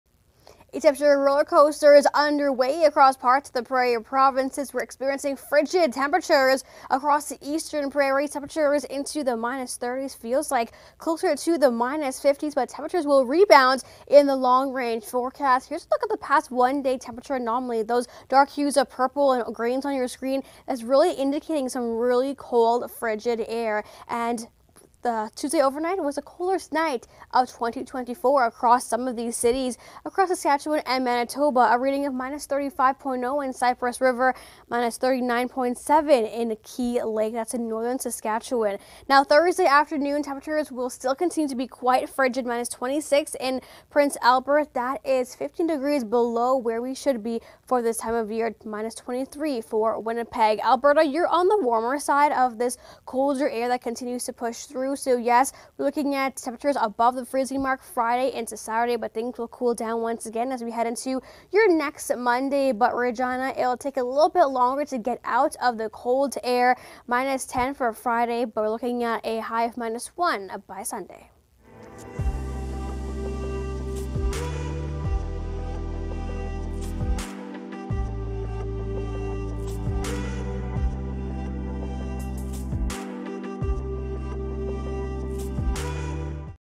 Temperature Roller Coaster: Dangerous Wind Chill Persists on the Prairies
Temperatures continue to plunge into the negative 30s, but a rebound is on the way. More details with meteorologist Melinda Singh.

Dangerous cold and wind chills lock onto the Prairies